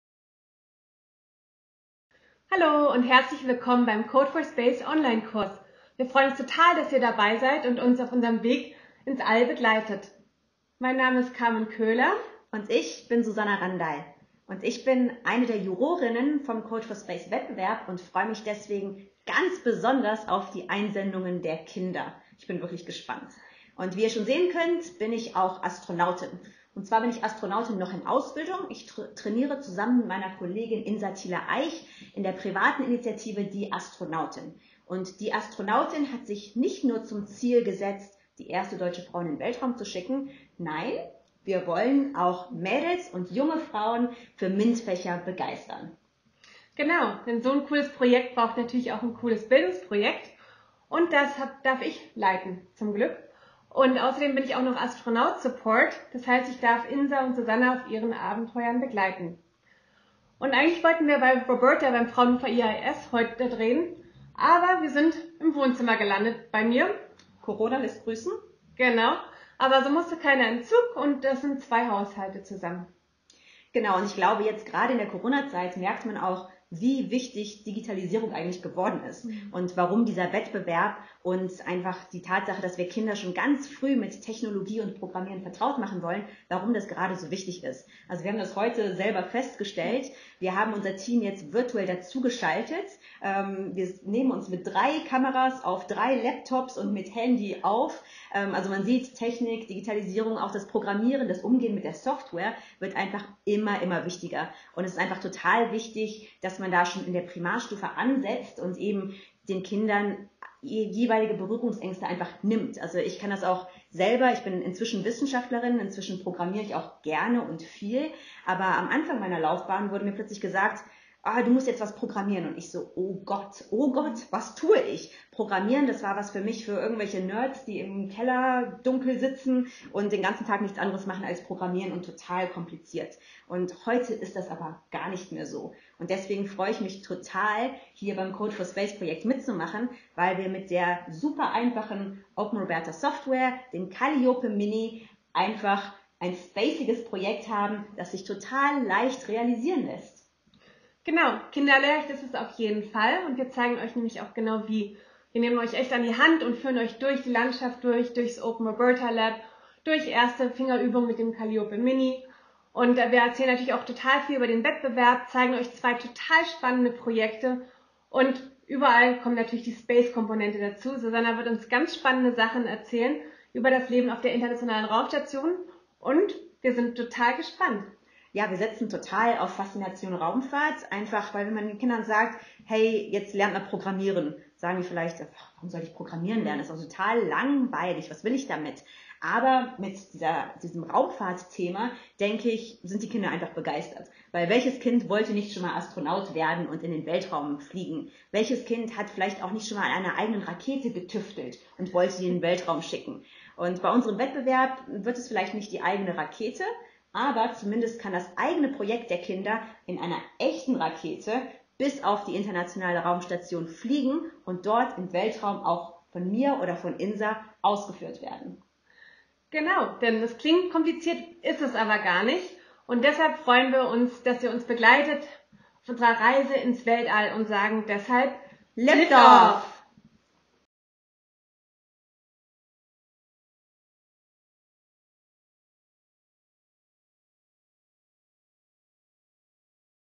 Let’s get started | Code4Space-Onlinekurs mit Die Astronautin | Teil 1
Im neuen Code4Space-Onlinekurs bieten die Roberta-Initiative von Fraunhofer IAIS und die Initiative erste deutsche Astronautin Euch einen umfassenden Einstieg ins Programmieren mit dem Open Roberta Lab und dem Calliope mini. Hier erhaltet Ihr nützliche Tipps und Praxisbeispiele für den Unterricht und die Teilnahme am Wettbewerb! Von Videos mit Beispielprojekten, Weltraum-Anekdoten und Hilfestellungen bis hin zu Lehr- und Lernmaterialien – der Code4Space-Onlinekurs bietet den idealen Einstieg für Lehrkräfte und Team-Mentor*innen, die gemeinsam mit ihren Schüler*innen am Wettbewerb teilnehmen und auf Weltraummission gehen wollen.

Das Code4Space-Team freut sich sehr, Euch bei diesem digitalen Weltraumabenteuer dabei zu haben. Dr. Carmen Köhler und Dr. Suzanna Randall von der Stiftung erste deutsche Astronautin führen Euch durch alle Abschnitte dieses Kurses und haben viel Know-how und Tipps parat.